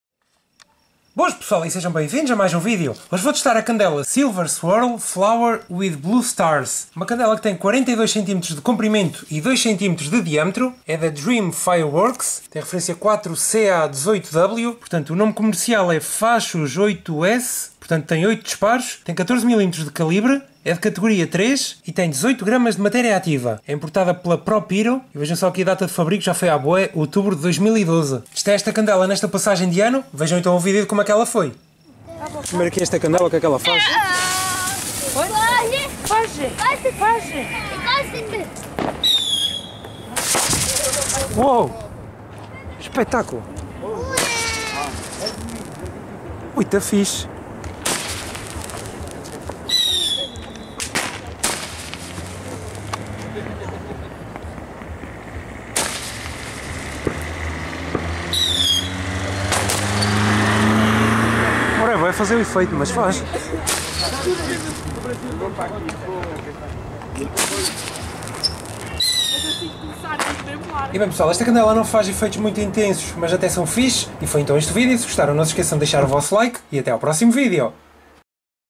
CANDELA SILVER SWIRL FLOWER WITH BLUE STARS - 8 DISPAROS
Boas pessoal!
Silenciosa e com efeitos espaçados.
🕒 40 segundos
💥 18 gr
Cat 3
Ø 14 mm
Ref 50015
Efeito:
↕ 42 cm
⟷ 2 cm
DREAM FIREWORKS
Propyro